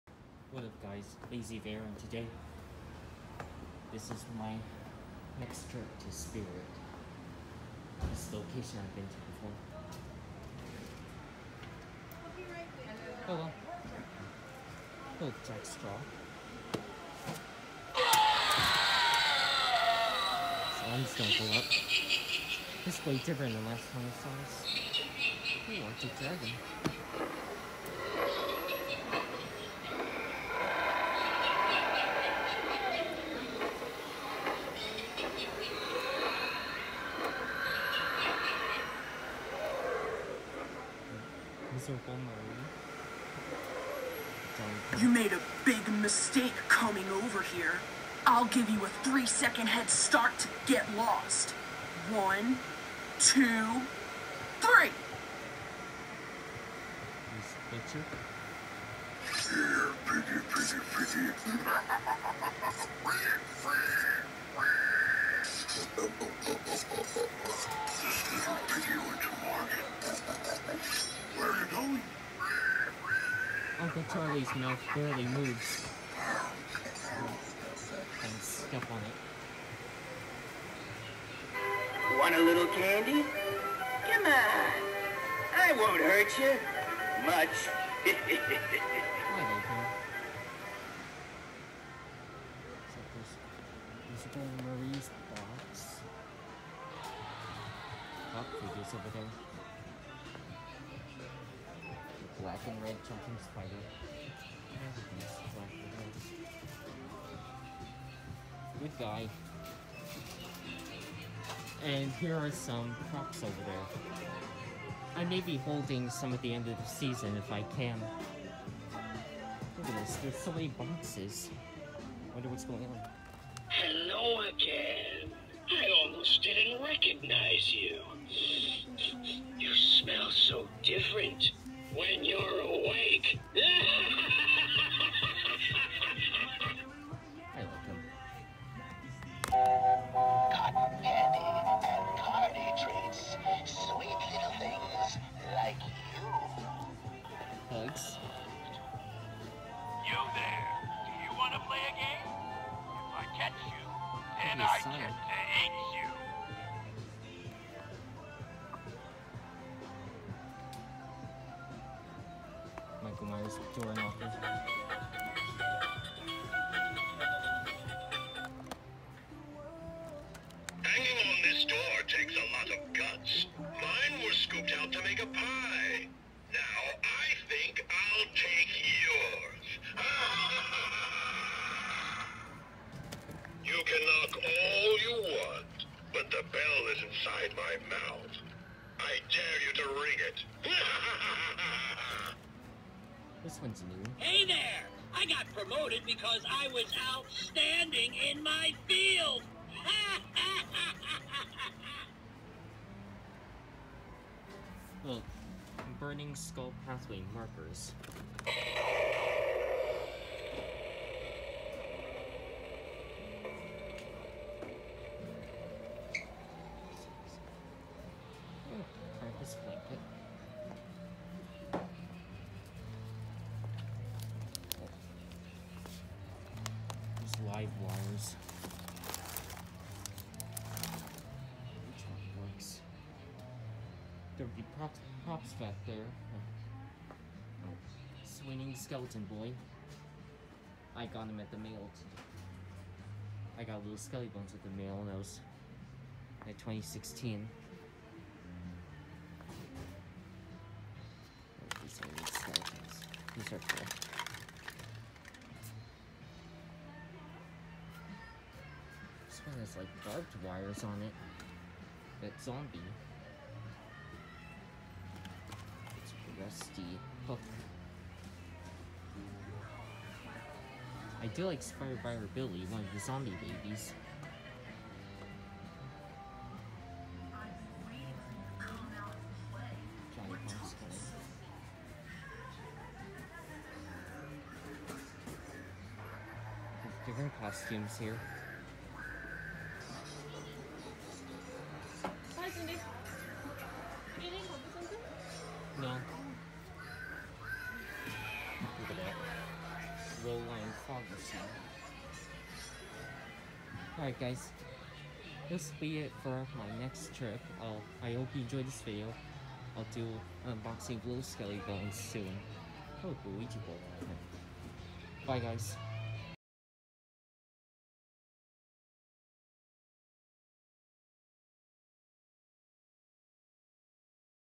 Spirit Halloween 2020 8th trip
This is my 8th trip to Spirit, and I wanted to hold some props up like Peek a boo clown & Fright in the box, but I didn’t see them in there. So I’ll go to another store to maybe get the display Peek a boo clown, and a few other things when it’s the %50 off sale.